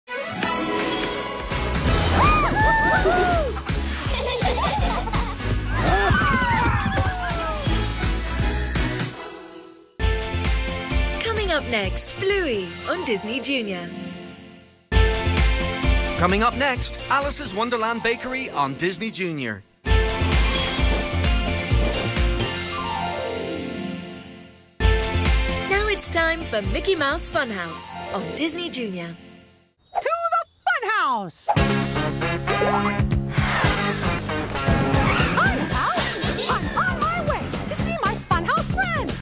Disney Junior HD Europe Magical Holidays Idents 2024🎄Christmas Bumpers - ENG
🎄Disney Junior HD Europe Magical Holidays Idents 2024 Christmas Bumpers - English